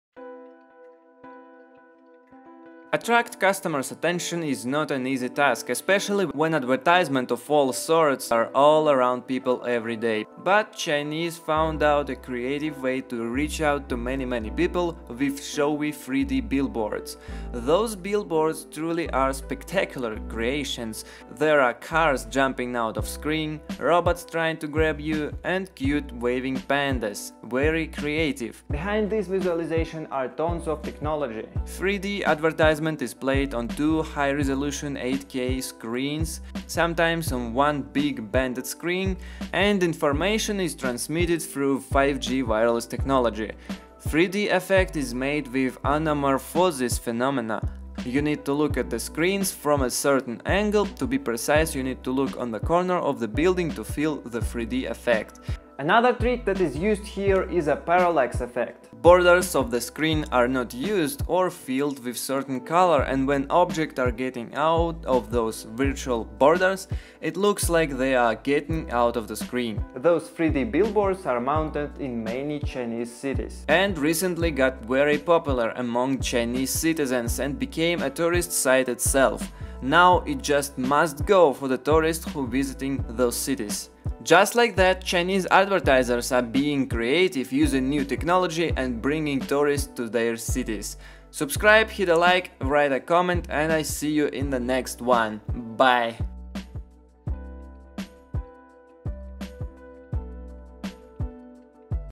Amazing 3D billboards in China
Here is some awesome 3D billboards from China and how are they made.